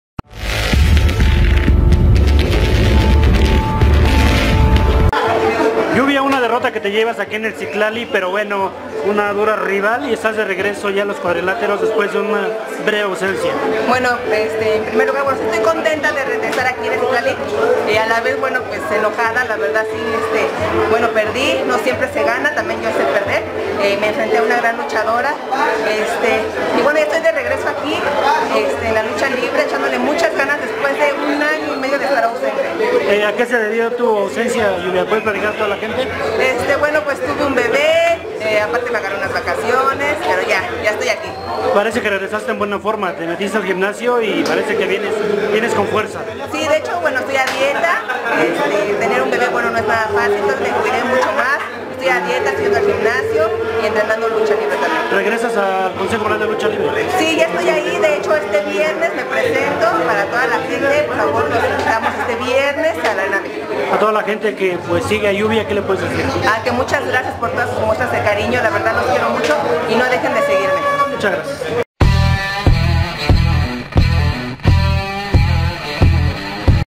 "Después de un año y medio de estar ausente, estoy de regreso" LLUVIA
Lluvia esta de regreso en la Lucha Libre después de un breve y obligado al ser madre, estas las declaraciones de la hija del Amo del Escándalo.